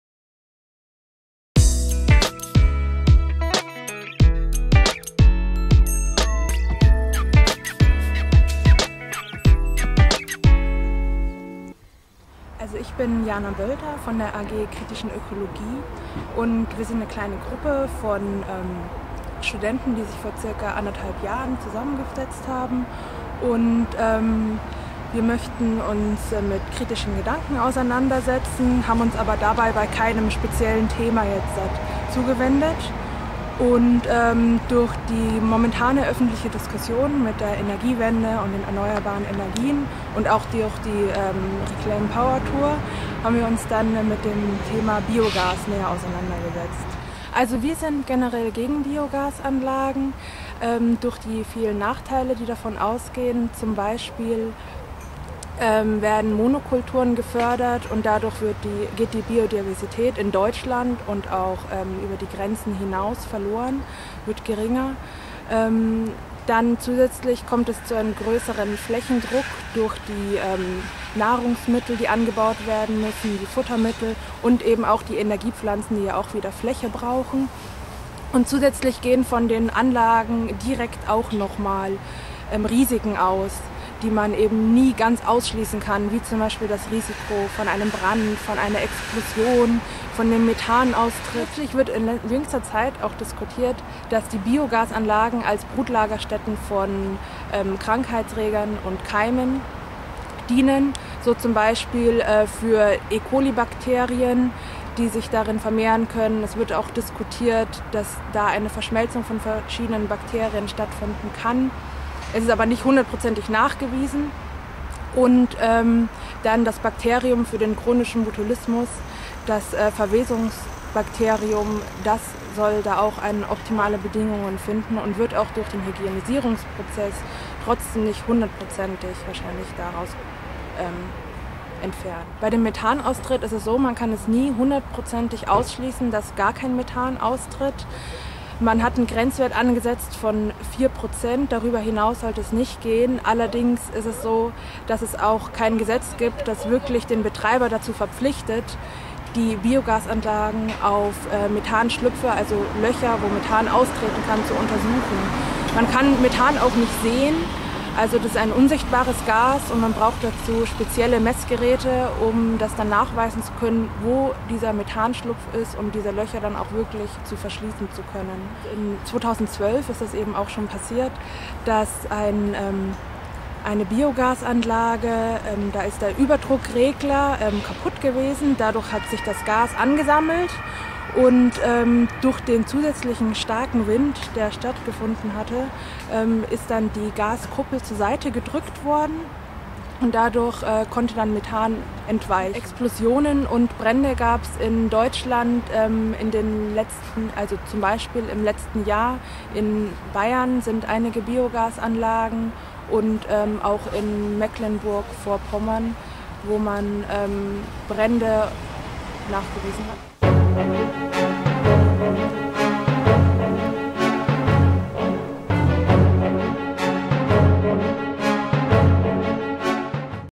ReclaimPowerTour Tag 25 - Münster: Biogaskritik
Zwischenstopp der Reclaimpowertour 2013 in Münster. AK Kritische Ökologie zum Thema Biogasanlagen. Fahrradtour vom Klimacamp in der Lausitz zum Klimacamp im Rheinland (Ost-West-Route) - mit vielen spannenden Vernetzungsstopps bei diversen Initiativen, wo Parallelen/Anregungen zum Energiethema gesehen werden. Für eine dezentrale Energiewende und Abkehr vom zerstörerischen System. Inklusive gesunder Kapitalismuskritik.